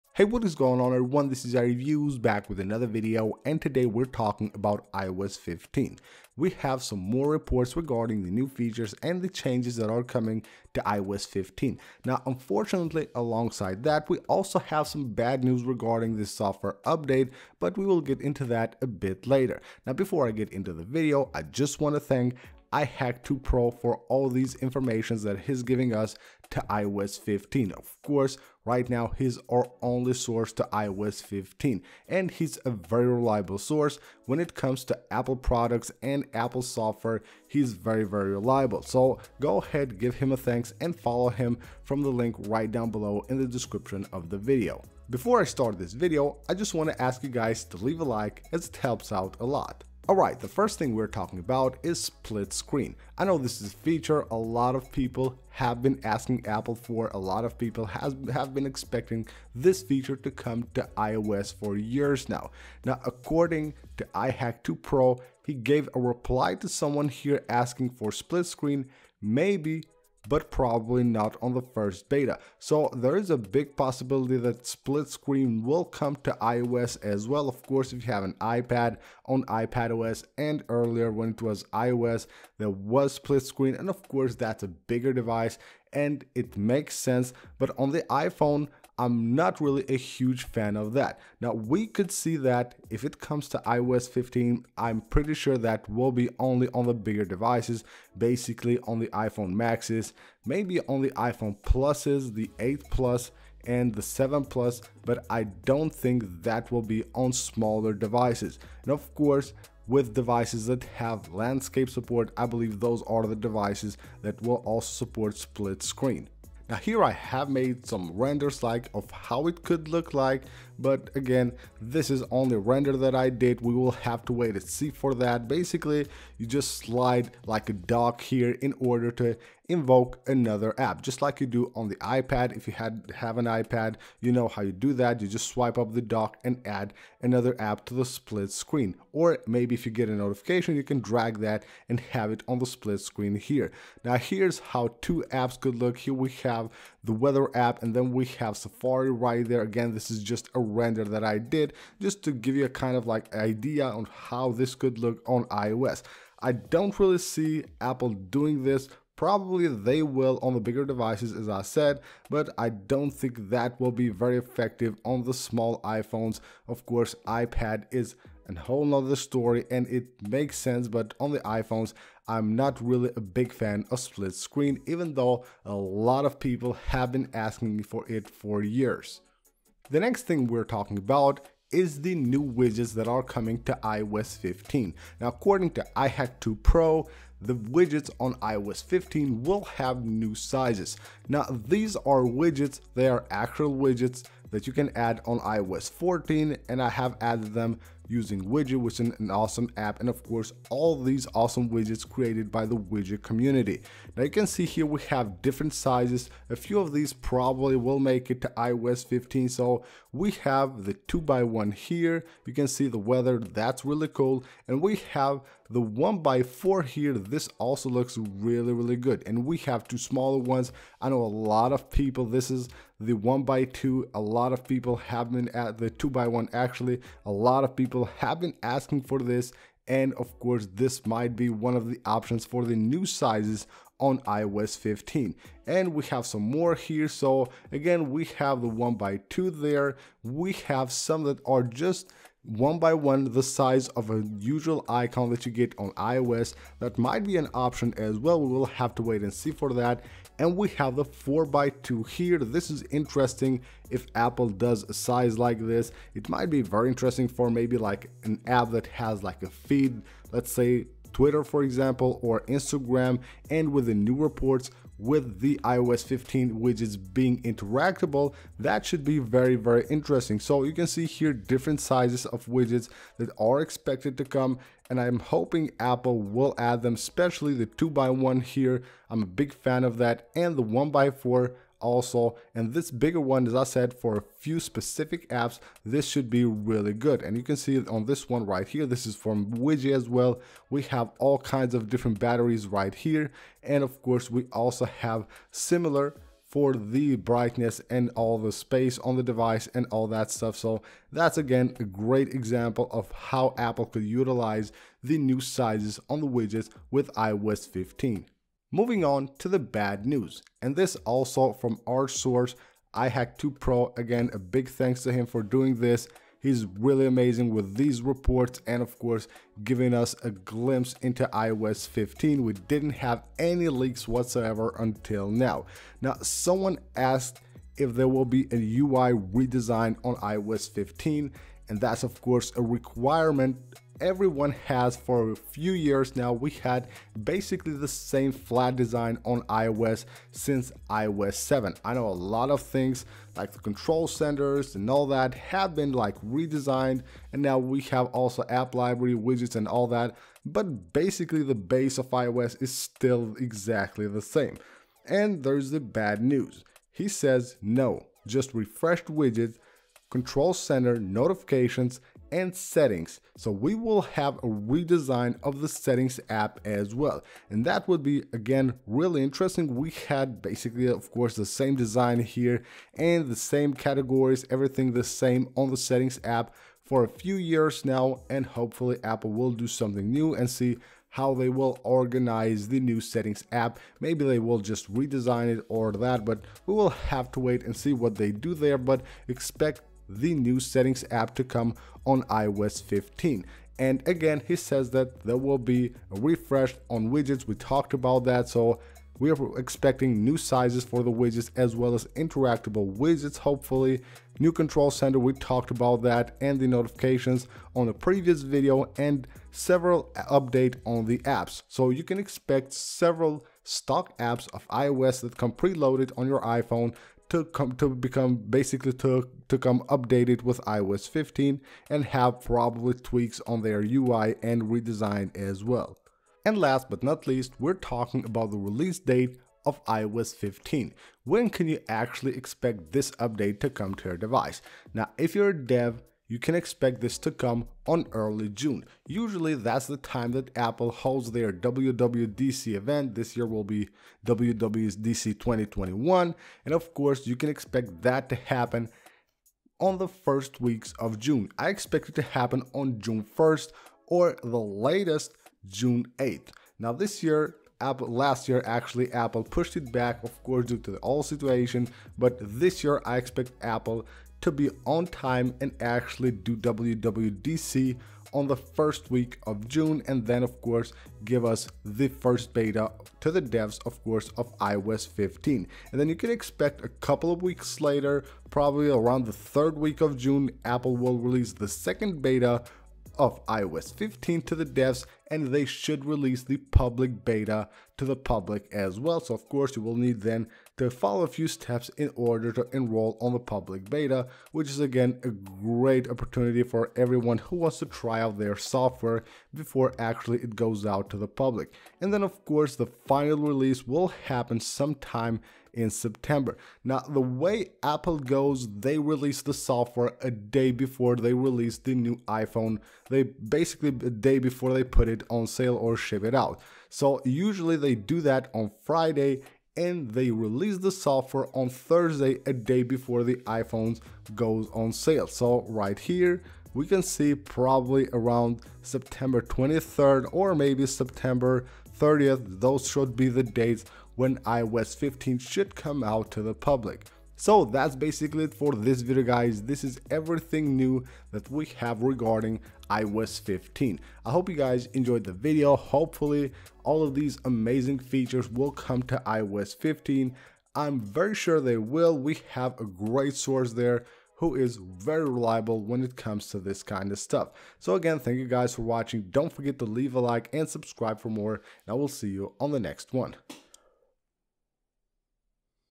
iOS 15 New Features LEAKED - Split Screen, Redesign & More !
iOS 15 new features have been leaked. iOS 15 new features have been reported. iOS 15 new features are coming with a lot of redesign and new UI changes.

A report shows us the new iOS 15 features that are expected to come to this years iOS 15 update, new features include a new Control Center, new widgets, split screen & more.